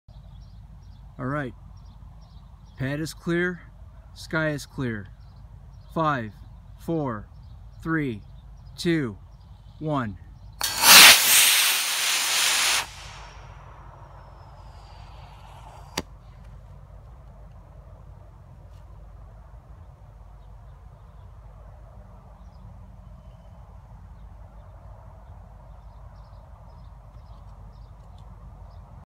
Estes Saturn V Launch
This is an Estes kit #2001.  It is a 1/100 scale model of the iconic Saturn V launch vehicle.  I've had this kit for over 30 years, but didn't have the confidence to build and launch it until now.  Altogether this rocket took 5 weeks to build.  The engine used was a D12-3.  Shortly after apogee I rotated my phone camera 90 degrees to keep the two descending sections in view, so apologies that it's disorienting.  The lower section has two parachutes, but only one opened.  Fortunately the rocket was recovered with no damage.  If you look carefully shortly after ejection you will see the last quarter moon sweep by.  It isn't the greatest camera recording, but I hope you enjoy the video.